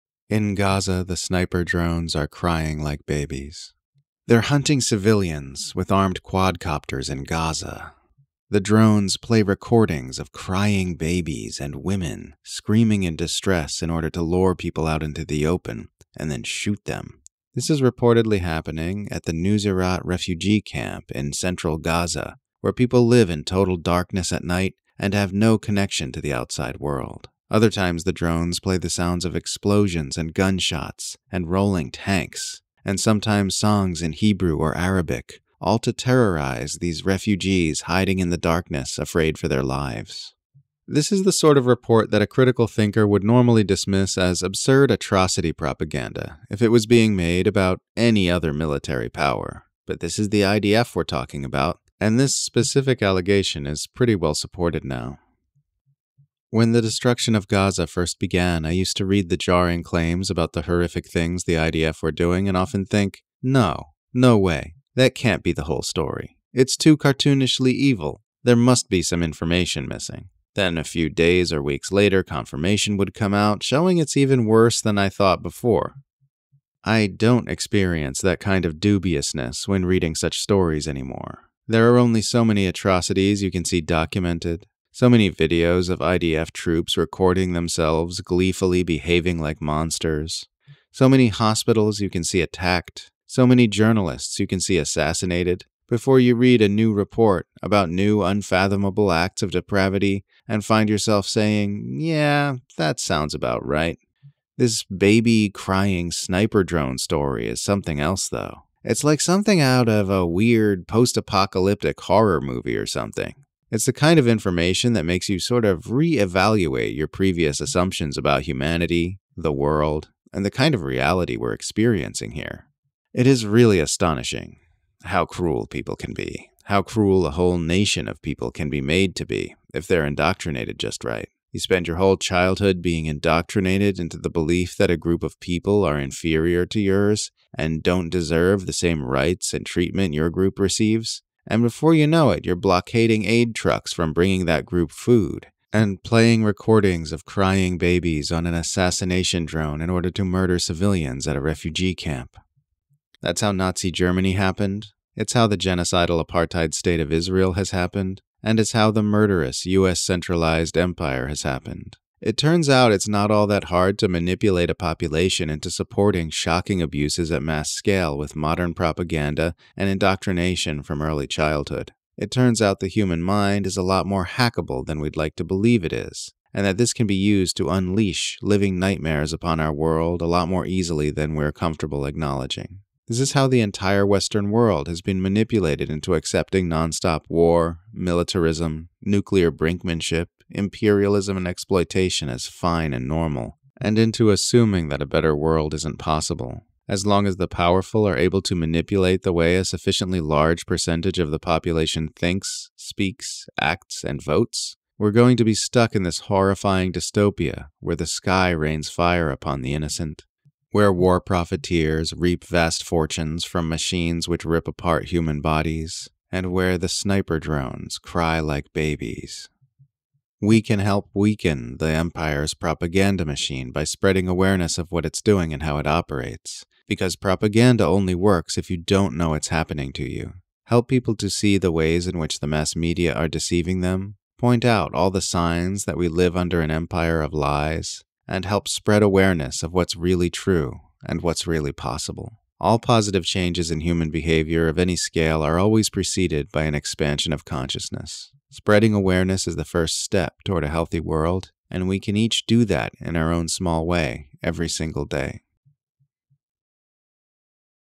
In Gaza The Sniper Drones Are Crying Like Babies
They're hunting civilians with armed quadcopters in Gaza.The drones play recordings of crying babies and women screaming in distress in order to lure people out into the open, and then shoot them.

Get on my free Substack newsletter caitlinjohnst.one to get all my content straight to your in-box.